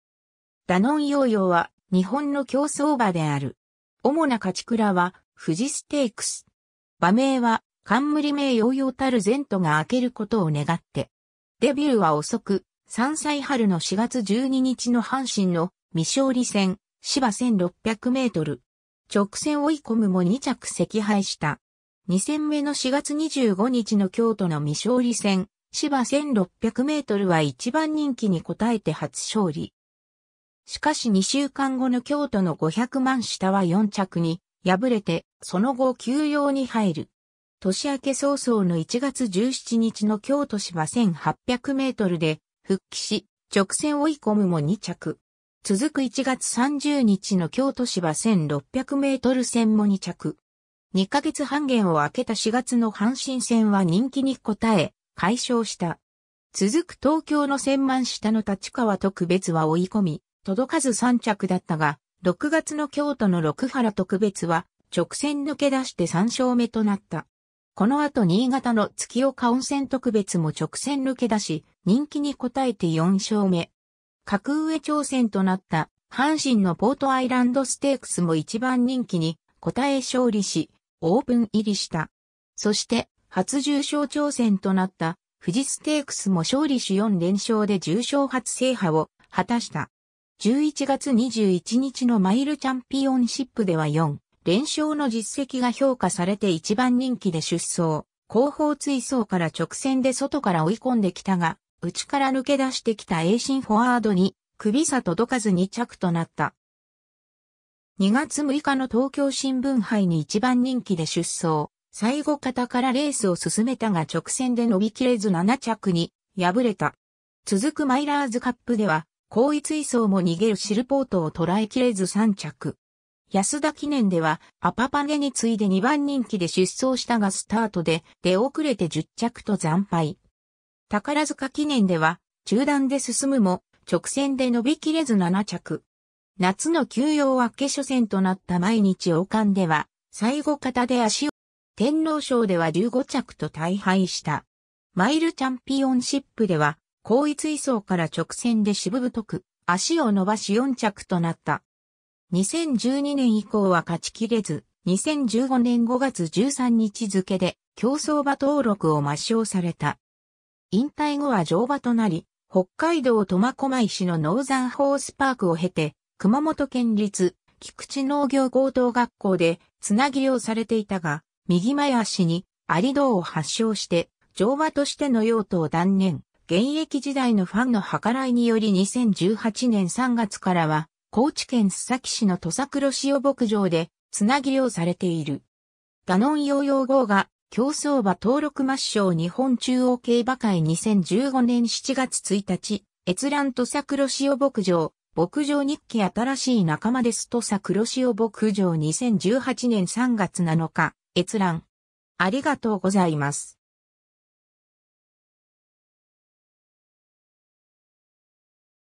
ダノンヨーヨー
ダノンヨーヨー (Danon Yoyo) は日本の競走馬である。
おもな勝ち鞍は富士ステークス。
馬名は冠名+洋洋たる前途が開けることを願って。
 デビューは遅く、3歳春の4月12日の阪神の未勝利戦芝1600m。
直線追い込むも2着惜敗した。
2戦目の4月25日の京都の未勝利戦芝1600mは1番人気に応えて初勝利。
しかし2週間後の京都の500万下は4着に敗れてその後休養に入る。
 年明け早々の1月17日の京都芝1800mで復帰し、直線追い込むも2着。
続く1月30日の京都芝1600m戦も2着。
2ヶ月半間を開けた4月の阪神戦は人気に応え快勝した。
続く東京の1000万下の立川特別は追い込み届かず3着だったが、6月の京都の六波羅特別は直線抜け出して3勝目となった。
このあと新潟の月岡温泉特別も直線抜け出し人気に応えて4勝目。
格上挑戦となった阪神のポートアイランドステークスも1番人気に応え勝利しオープン入りした。
そして初重賞挑戦となった富士ステークスも勝利し4連勝で重賞初制覇を果たした。
11月21日のマイルチャンピオンシップでは4連勝の実績が評価されて1番人気で出走、後方追走から直線で外から追い込んできたが、内から抜け出してきたエーシンフォワードにクビ差届かず2着となった。
 2月6日の東京新聞杯に1番人気で出走、最後方からレースを進めたが直線で伸び切れず7着に敗れた。
続くマイラーズカップでは好位追走も逃げるシルポートをとらえきれず3着。
安田記念ではアパパネに次いで2番人気で出走したがスタートで出遅れて10着と惨敗。
宝塚記念では中団で進むも直線で伸び切れず7着。
夏の休養明け緒戦となった毎日王冠では最後方で脚を溜めたが直線で伸びず6着。
天皇賞（秋）では15着と大敗した。
マイルチャンピオンシップでは好位追走から直線で渋太く脚を伸ばし4着となった。
 2012年以降は勝ちきれず、2015年5月13日付けで競走馬登録を抹消された。
引退後は乗馬となり、北海道苫小牧市のノーザンホースパークを経て熊本県立菊池農業高等学校で繋養されていたが、右前脚に蟻洞を発症して乗馬としての用途を断念、現役時代のファンの計らいにより2018年3月からは高知県須崎市の土佐黒潮牧場で繋養されている。
 ^ ダノンヨーヨー号が競走馬登録抹消–日本中央競馬会　2015年7月1日閲覧 ^ 土佐黒潮牧場「牧場日記」 新しい仲間です♪–土佐黒潮牧場　2018年3月7日閲覧